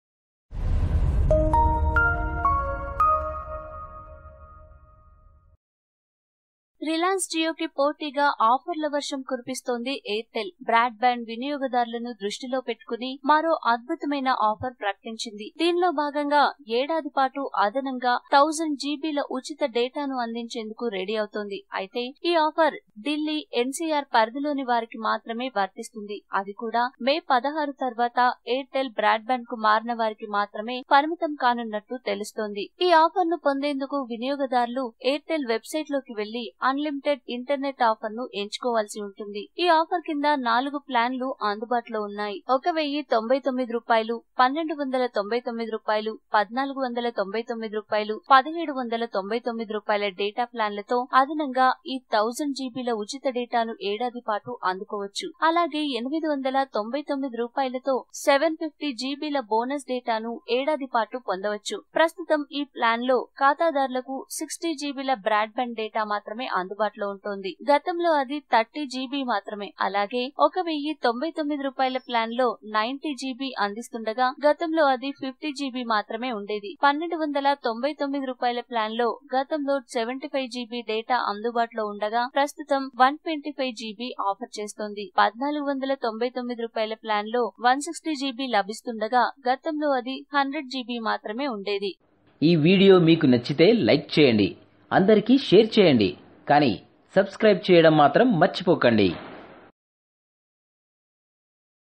AMAZING! Airtel to Provide 1000 GB FREE Data | Mobile Networks | Latest Updates | VTube Telugu
AMAZING! Airtel to Provide 1000 GB FREE Data. Check out the full details about Airtel Data Offers in this video on VTube Telugu.